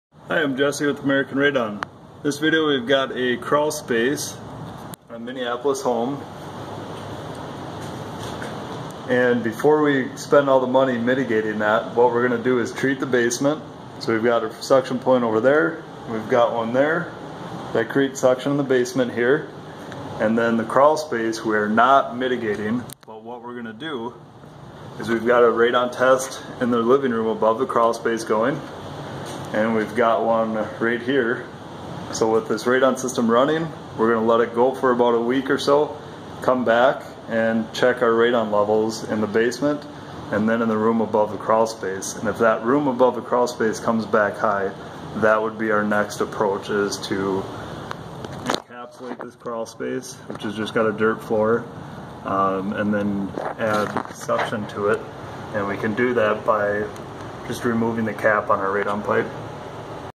Crawl Space Radon: Why We Don’t Always Encapsulate
We prefer a phased approach before encapsulating a crawl space with a dirt floor like this one.

Often, once we mitigate the lowest level of the home, there’s no need to address the crawl space at all. In this case, we designed and installed a radon system for the basement, then tested radon levels in both the basement and the room above the crawl space.

*Post-Mitigation Radon Test Results*
Basement test: 0.8 pCi/L
Living room above crawl space: 0.4 pCi/L

Since both results were low, we recommended that the homeowner continue monitoring each area using long‑term radon test kits, rather than moving straight to crawl space encapsulation.

While encapsulating a crawl space can improve indoor air quality, it may not be the main pathway for radon entry in every home. This approach can save you money while still keeping your radon levels in check.